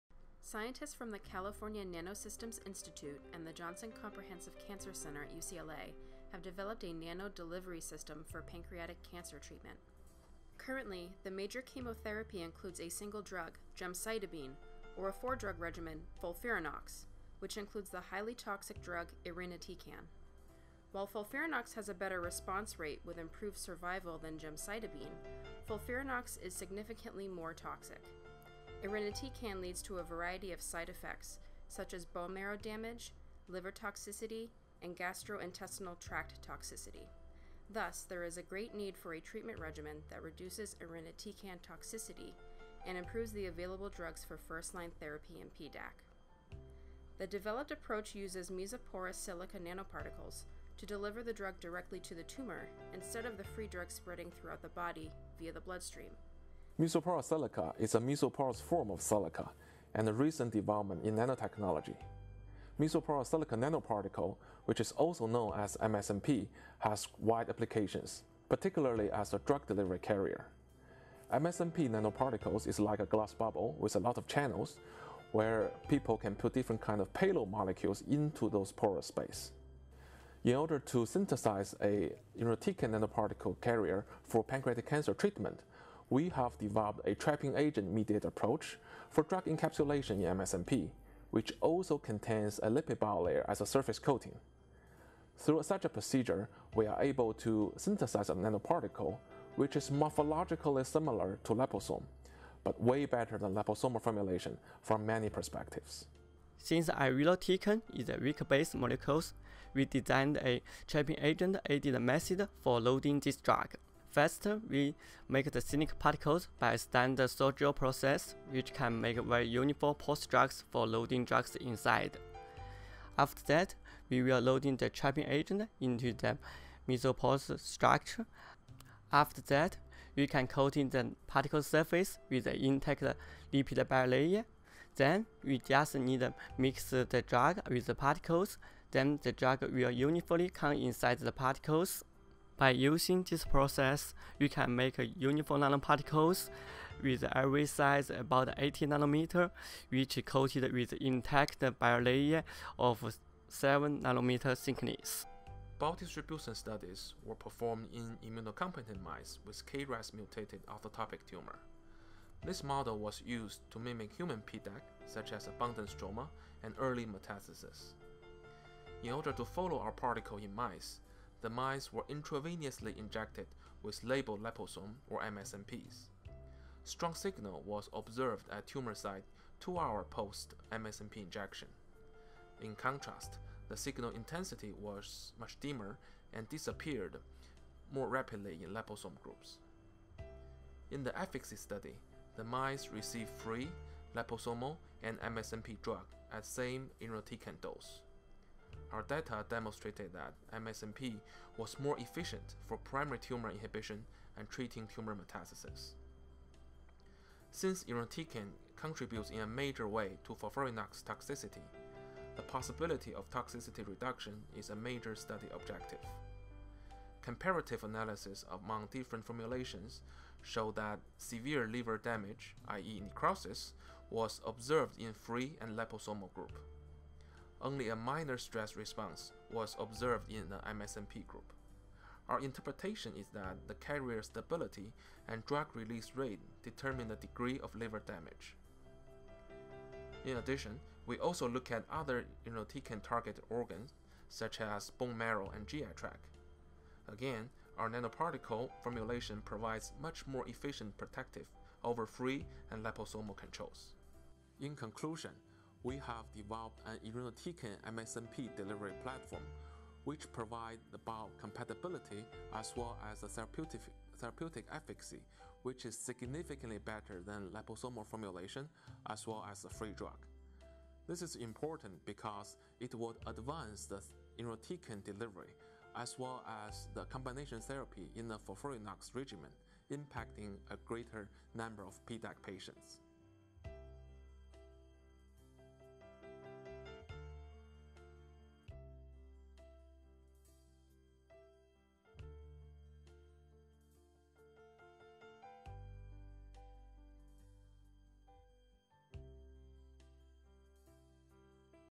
Nano-enabled chemotheraphy for pancreatic cancer with improved safety and efficacy
Authors Huan Meng and Xiangsheng Liu describe the custom-designing of a mesoporous silica nanoparticle (MSNP) platform to reduce the toxicity of irinotecan, a drug included in the four-drug regimen FOLFIRINOX, to treat pancreatic ductal adenocarcinoma (PDAC).